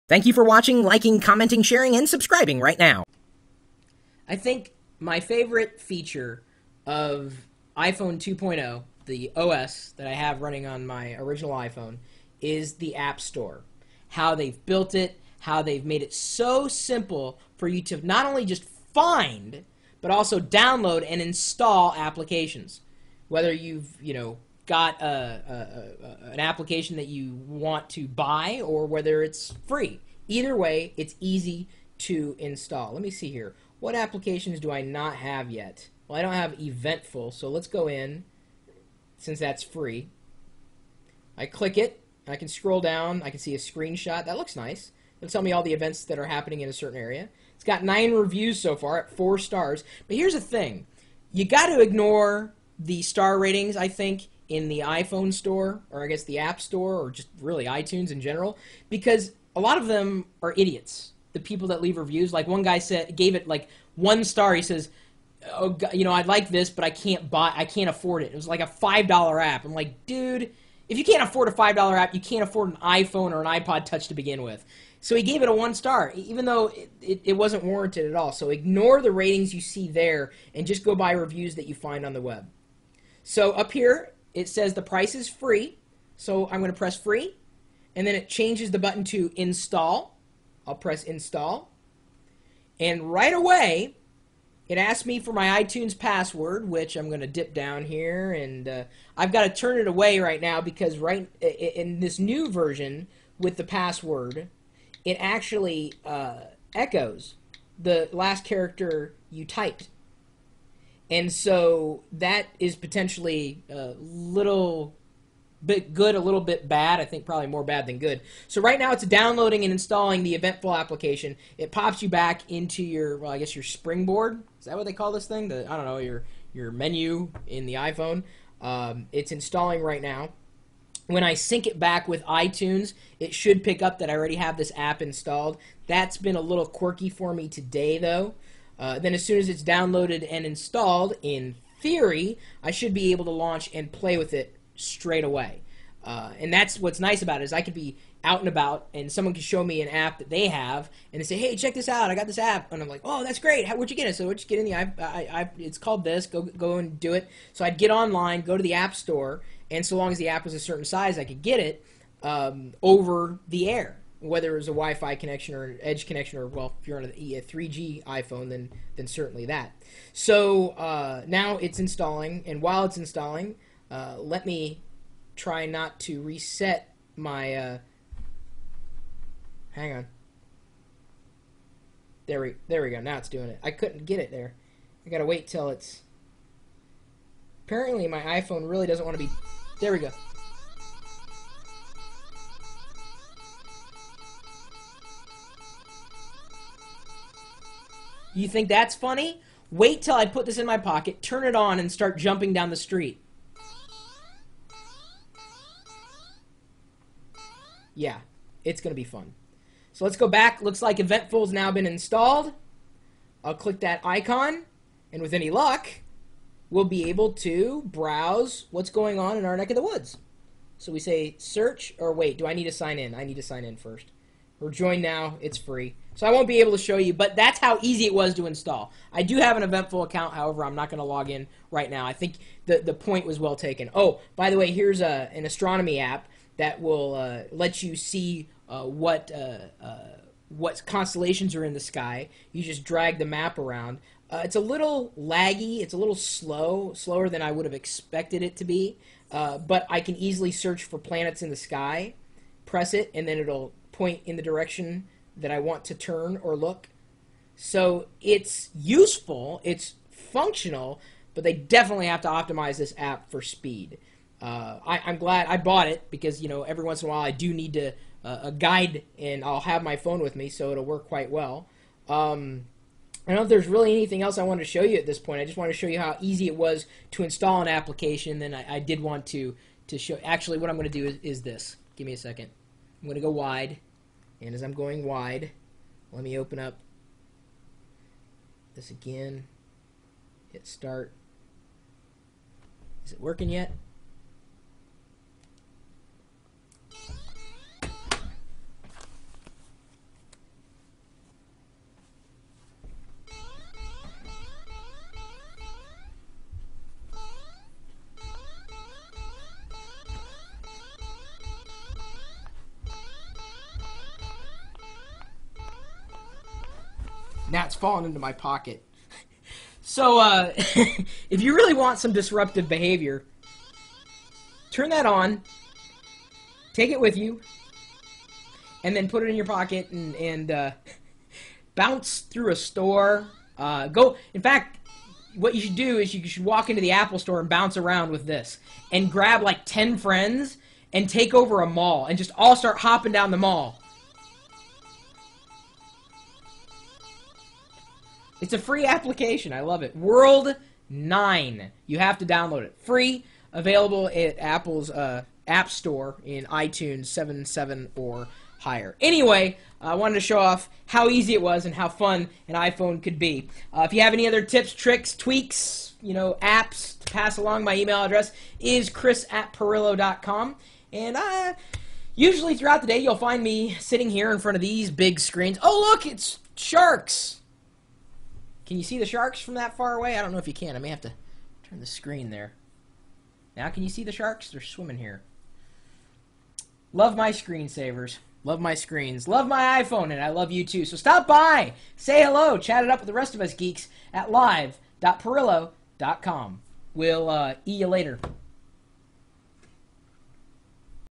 How do You Install Apps on the iPhone?
- My favorite feature of iPhone 2.0 operating system has to be the App Store. I love the design, and how easy it is to find and download whatever you're looking for. They're even simple to install.